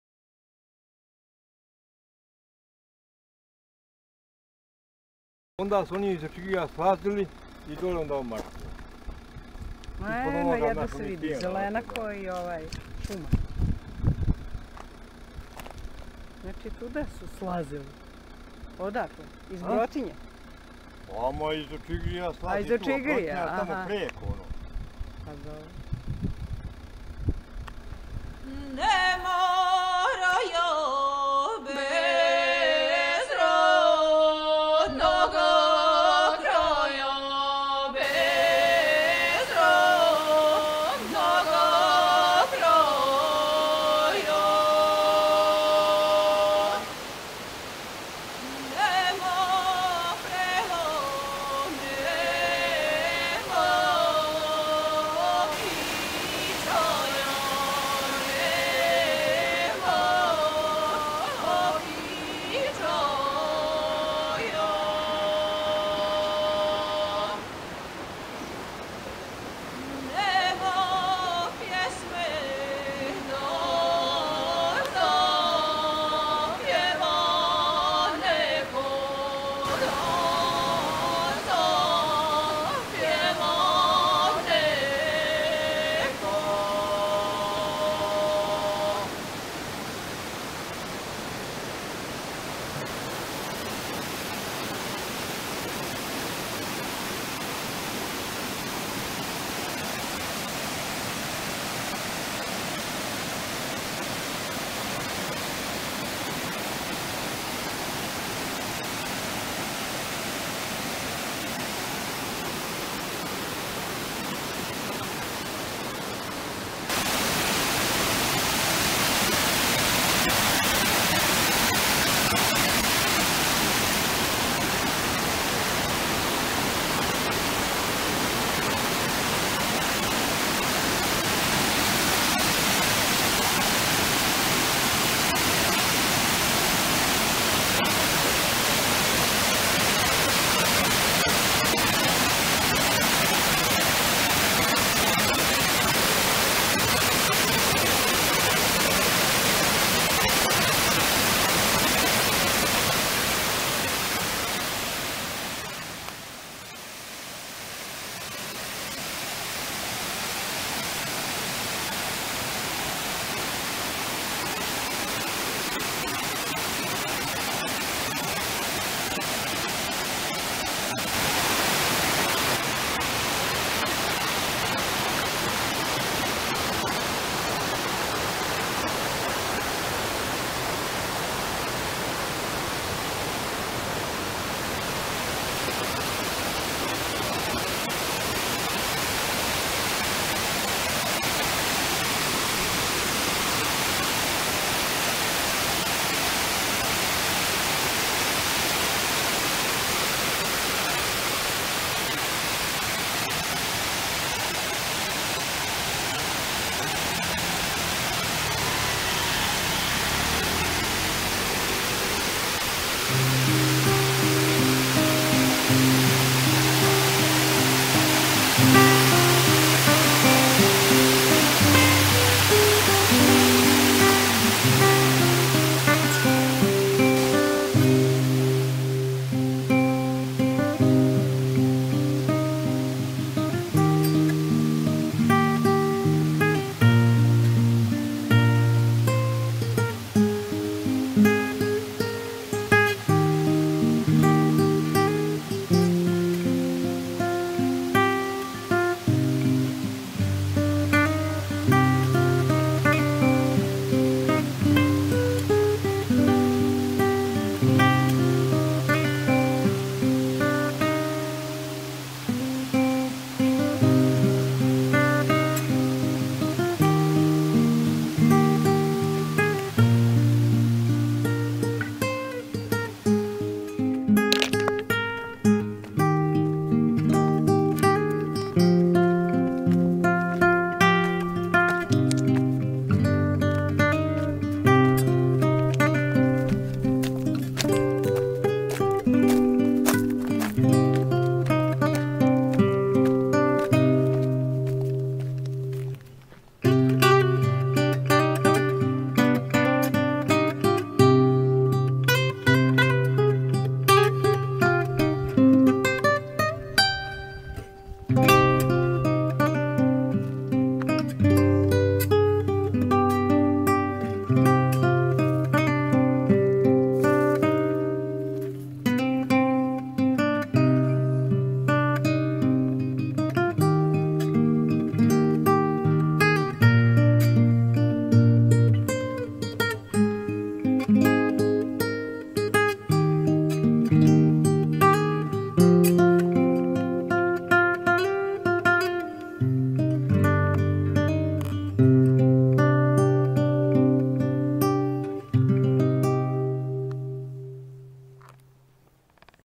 Мартин Брод - Уна кипти
водопади и брзаци Уне и станица у Мартином Броду - август 2019.